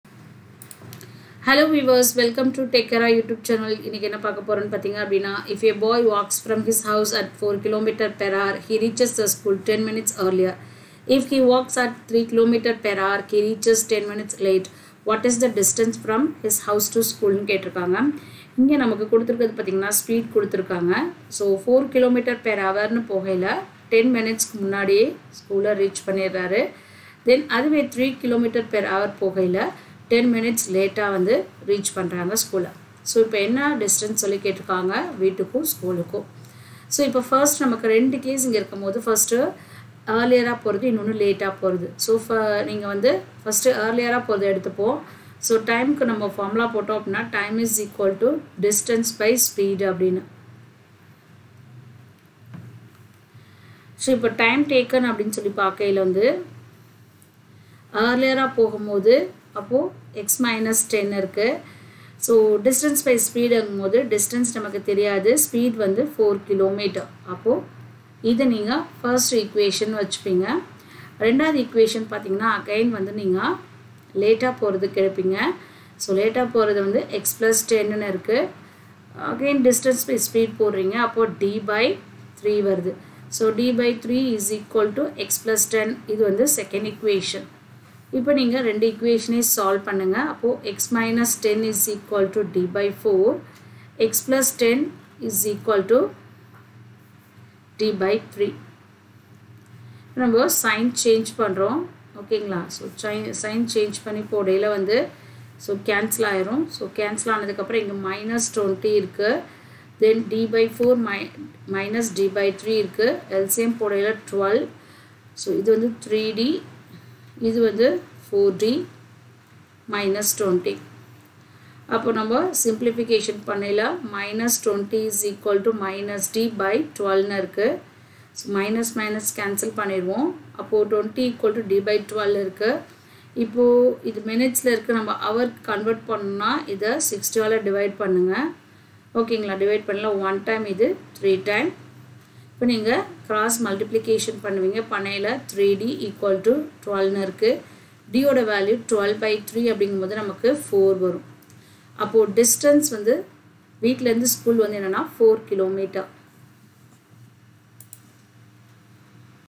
If a boy walks from his house at 4 km/hr, he reaches school 10 minutes early | Tamil
If a boy walks from his house at 4 km/hr, he reaches school 10 minutes early. If he walks at 3 km/hr, he reaches 10 minutes late. What is the distance from his house to school ?
(A) 6 km (B) 4 km